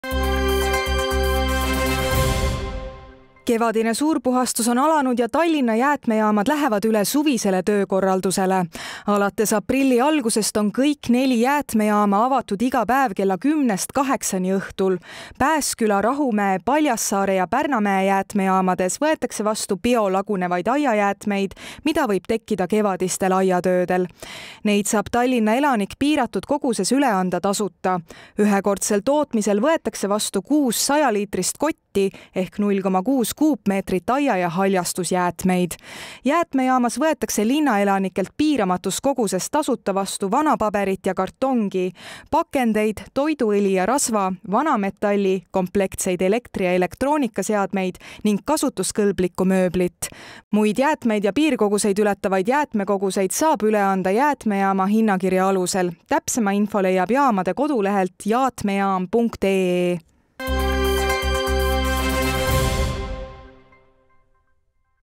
30.03.2022 - TALLINNA JÄÄTMEJAAMAD ON 1. APRILLIST AVATUD IGA PÄEV
Kevadine suurpuhastus on alanud ja  Tallinna Jäätmejaamad lähevad üle suvisele töökorrlaldusele. Alates aprilli algusest on kõik neli jäätmejaama avatud iga päev kella 10 - kaheksaani õhtul.
Pääsküla, Rahumäe, Paljassaare ja Pärnamäe jäätmejaamades võetakse vasu
Biolagunevaid aiajäätmeid, mida võib tekkida kevadistel aiatöödel. Neid  saab Tallinna elanik piiratud koguses üle anda tasuta. Ühekordsel toomisel võetakse vastu kuus sajaliitrist kotti ehk 0,6 kuupmeetrit aia- ja haljastusjäätmeid.
Jäätmejaamas võetakse linnaelanikelt piiramatus koguses tasuta vastu vanapaberit ja kartongi, pakendeid, toiduõli ja -rasva, vanametalli, komplektseid elektri- ja elektroonikaseadmeid ning kasutuskõlblikku mööblit.
Muid jäätmeid ja piirkoguseid ületavaid jäätmekoguseid saab üle anda jäätmejaama hinnakirja alusel. Täpsema info leiab jaamade kodulehelt jaatmejaam.ee.